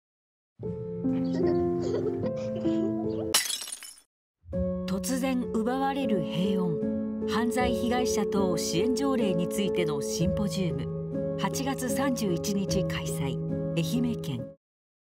「愛媛県犯罪被害者等支援条例」制定記念シンポジウム告知（愛媛県主催）
愛媛県では、犯罪被害に遭われた方の被害の回復や軽減、生活再建はもとより、
みんなで支え合いながら誰もが安心して暮らすことができる社会の実現に向け、
「愛媛県犯罪被害者等支援条例」を制定しました。
条例には基本理念や二次被害防止などを定めています。
“よりそうために知ること”が大切です。
被害に遭われた方やそのご家族、そして県民の皆さまが
シンポジウムを通じてそれぞれの立場で理解を深めませんか。

　≪シンポジウム概要≫
【日時】令和5年8月31日（木）14時～16時20分
【内容】愛媛県犯罪被害者等支援条例の説明
講演①：愛知県西尾市女子高生ストーカー殺人事件ご遺族　永谷博司さん
「高校2年生（16歳）で短い命を落とした娘との思い出と悔しさ」
講演②：横浜市社会福祉職・犯罪被害者相談室相談員　木本克己さん
「犯罪被害者等支援について考える」
愛媛県警察音楽隊によるミニコンサート　など
【場所】愛媛県男女共同参画センター多目的ホール（松山市山越町450番地）
【定員】250人（参加無料）
【応募期間】8月17日（木）まで